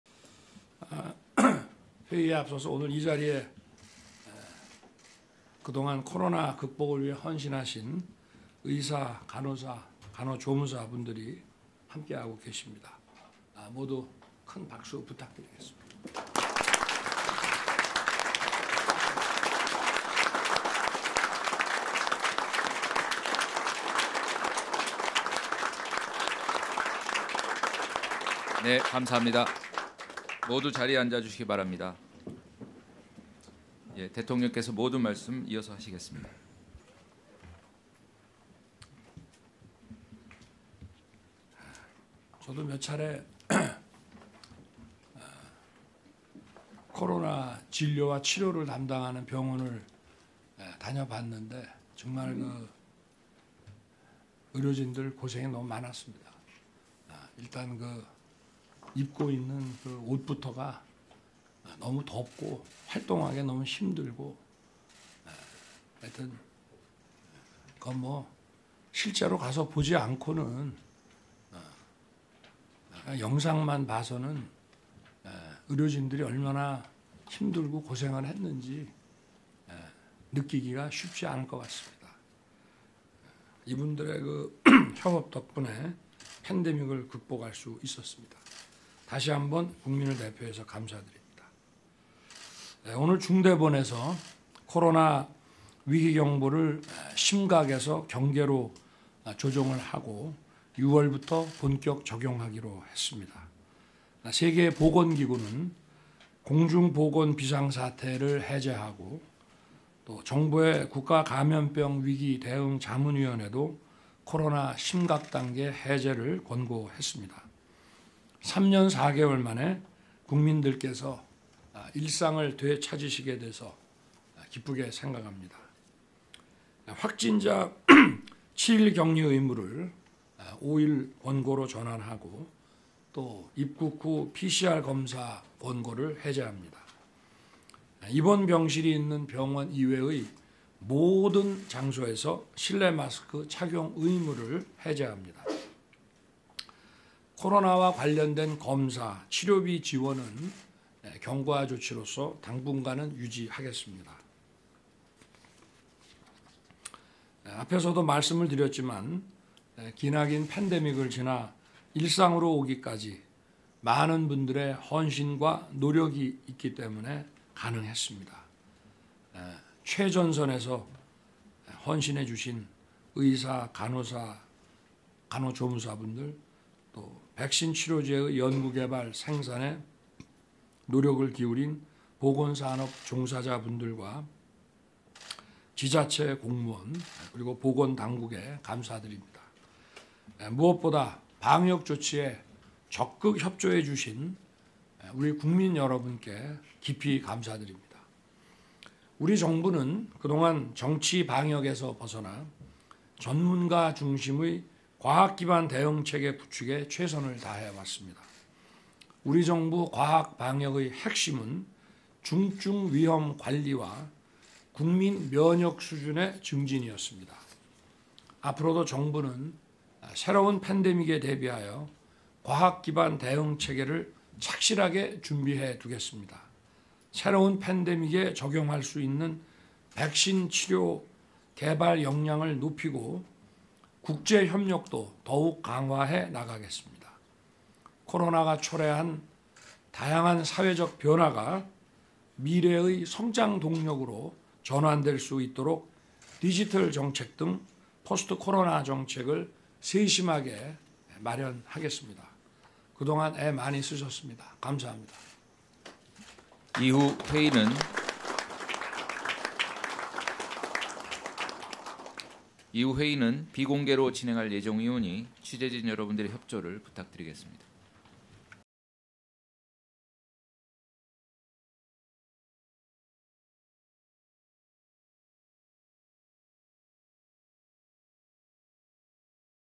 [현장영상] "3년 4개월만에 일상 회복으로".. 코로나19 중대본 윤석열 대통령 발언 / OBS 뉴스
"3년 4개월만에 일상 회복으로".. 코로나19 중대본 윤석열 대통령 발언